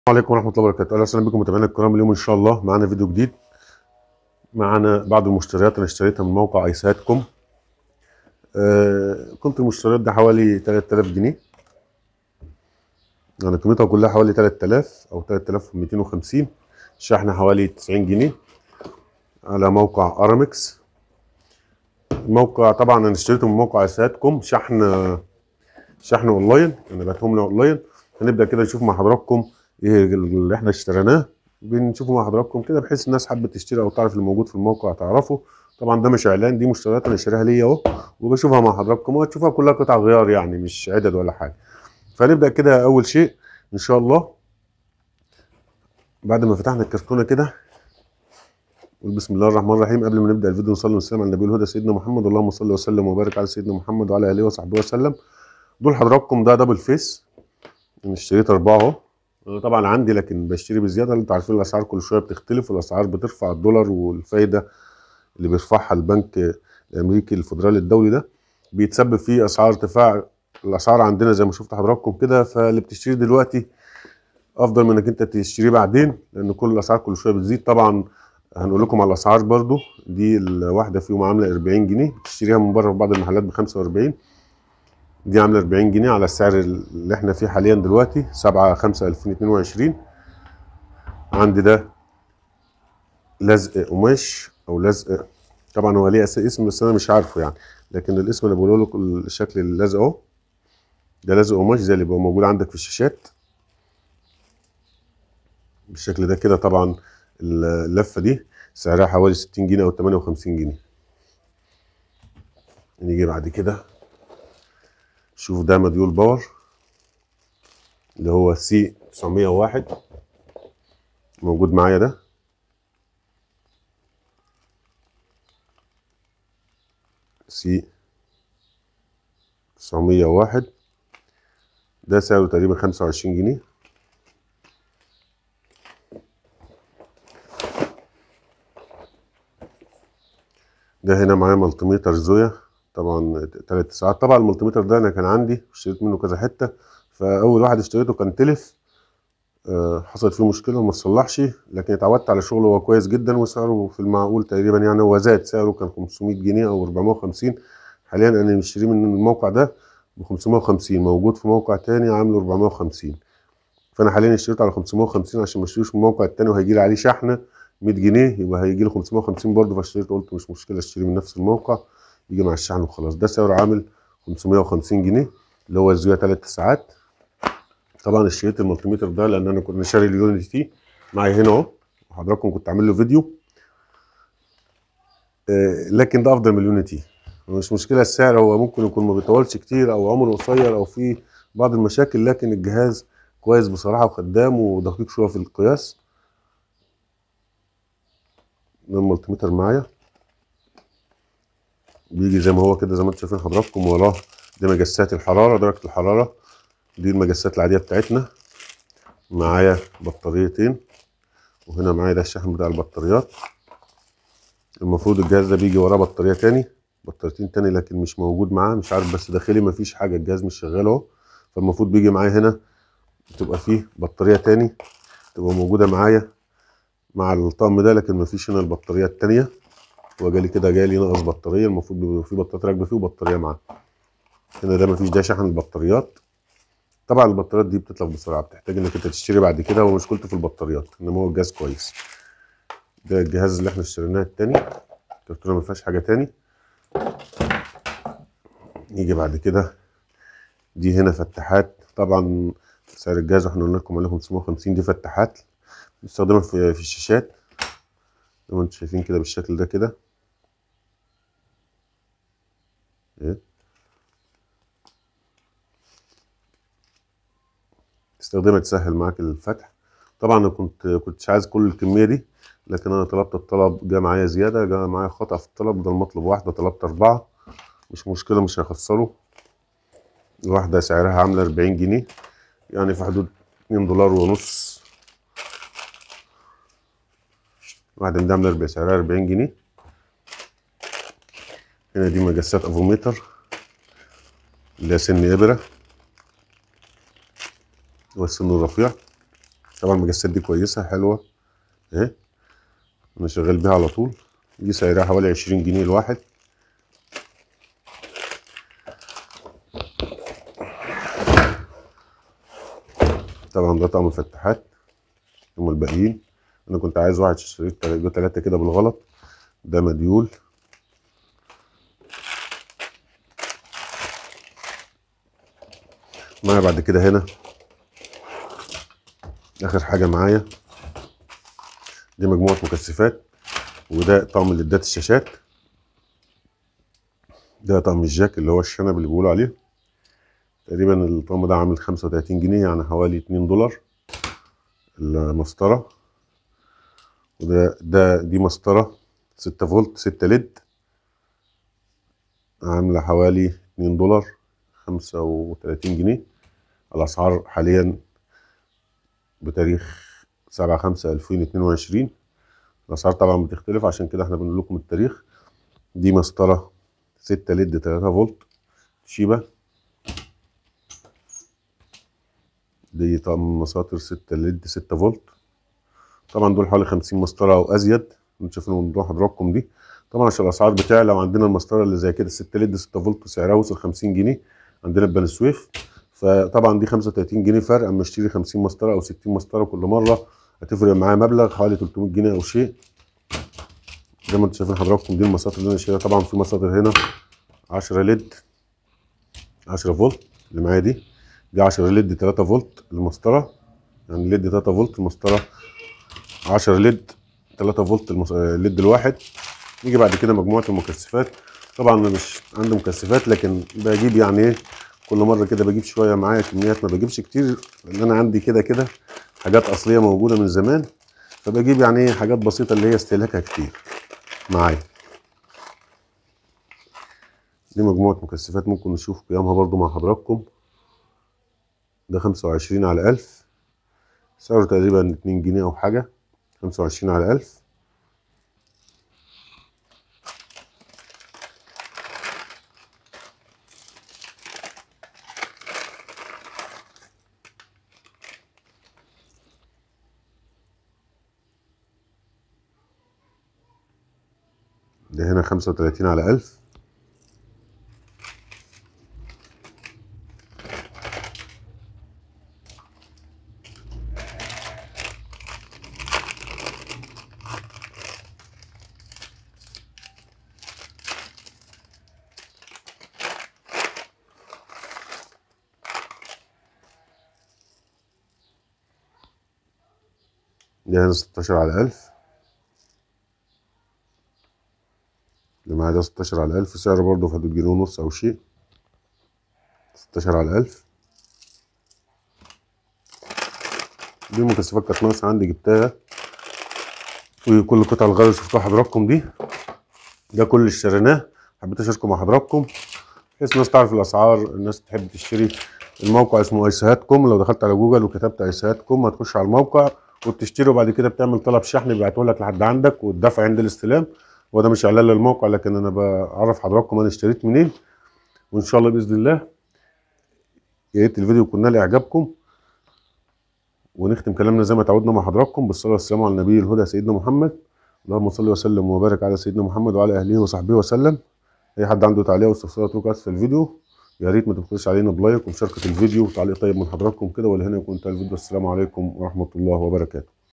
بعض مشترياتي من قطع الغيار واخر سعر بعد ارتفاع الدولار 7/5/2022
بعض مشترياتي من قطع الغيار واخر سعر بعد ارتفاع الدولار  رابط الموقع..
■من اراد دعم القناة ماديا ودعمي لتطوير القناة
●باي بال pay pal
☆☆☆☆(بعض اهم فديوهات القناة)☆☆☆☆
طرق تساعدك في احتراف صيانة المازربورد عطل لمبة حمراء شاشة اسمارت
https: